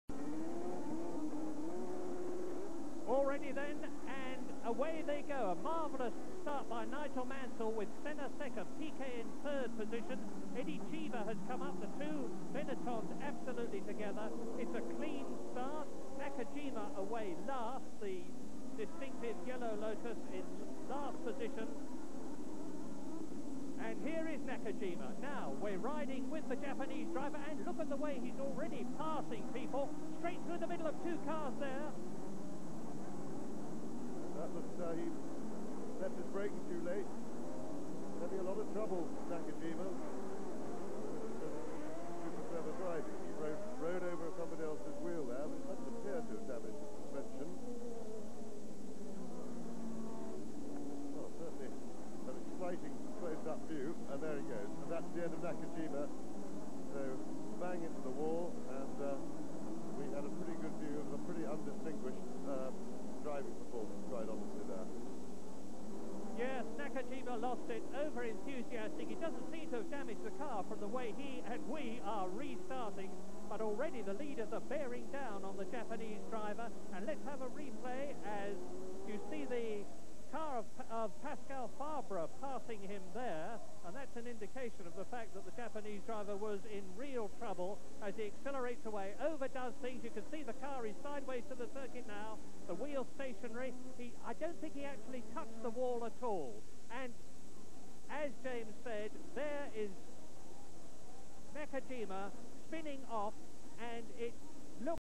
1987 - Detriot - Onboard with Satoru Nakajima
Part the onboard series with Satoru Nakajima in his Lotus-Honda car broadcasted by the BBC.

Circuit: Detriot at the 1987 United States Grand Prix.